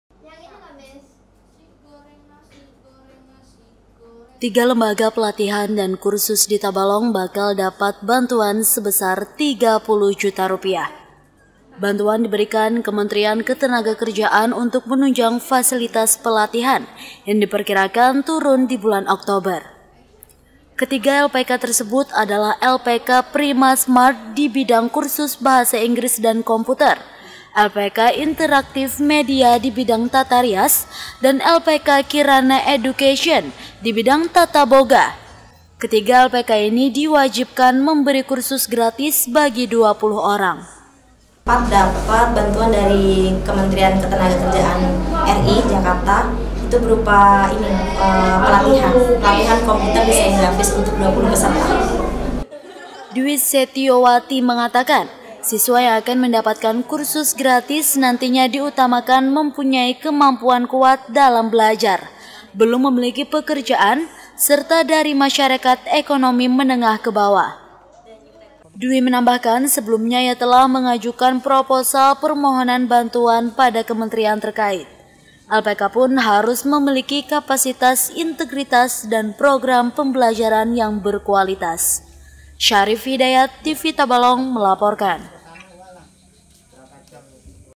3 LPK Dapat Bantuan Kursus Gratis Yang Diperuntukkan Masyarakat Kurang Mampu
Tiga Lembaga Pelatihan dan Kursus (LPK) di Tabalong, bakal mendapat bantuan penunjang pelatihan sebesar masing-masing Rp 30 juta. LPK yang mendpaat bantuan tersebut, diwajibkan untuk berikan kursus gratis, bagi 20 orang masyarakat dengan ekonomi lemah.